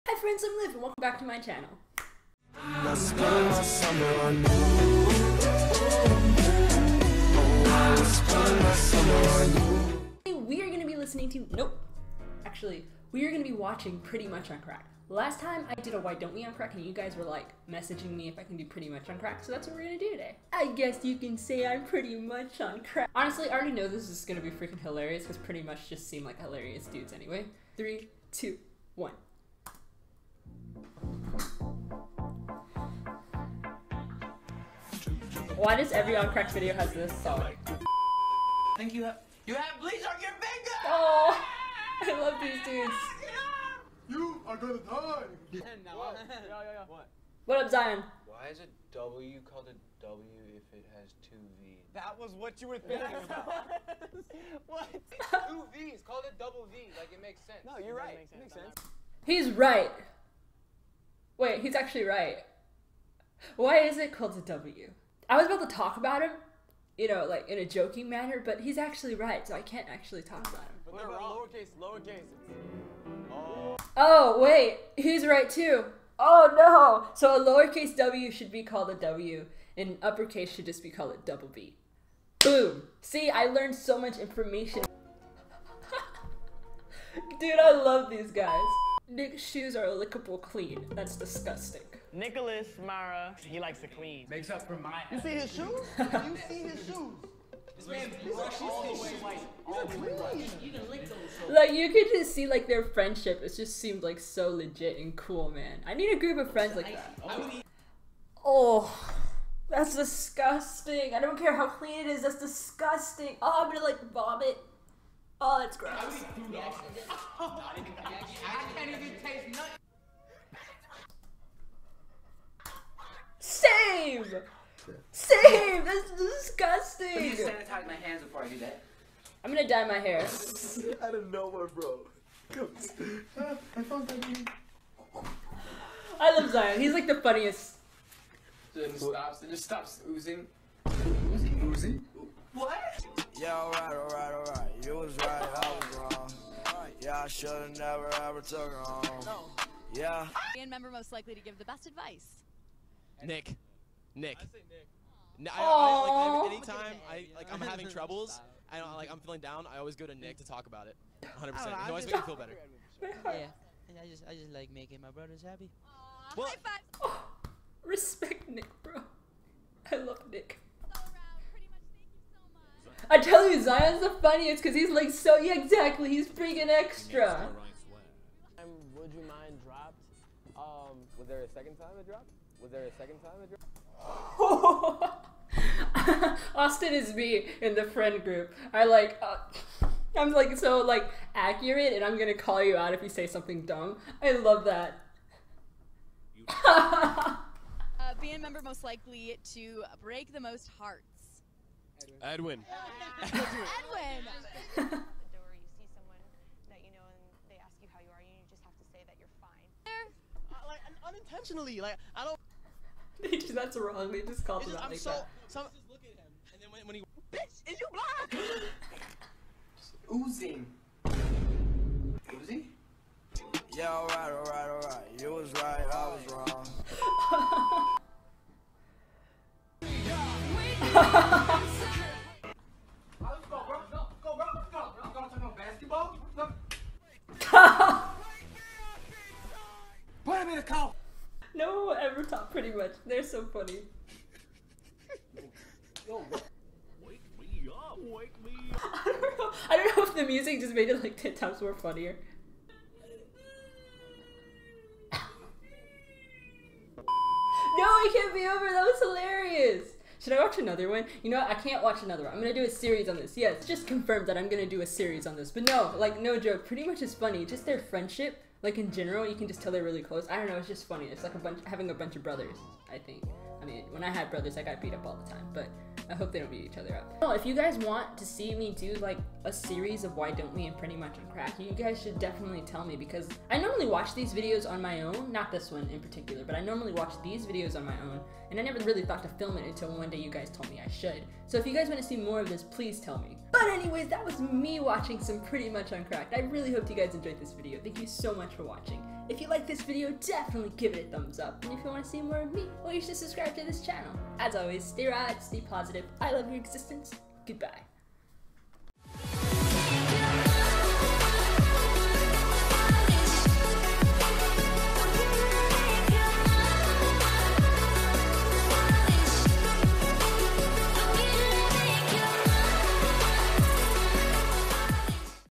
PrettyMuch On Crack Reaction! | ZION IS SO EXTRA
So after my Why don't We on crack video you humans have been asking me to watch PrettyMuch on crack! SO HERE WE ARE ENJOY!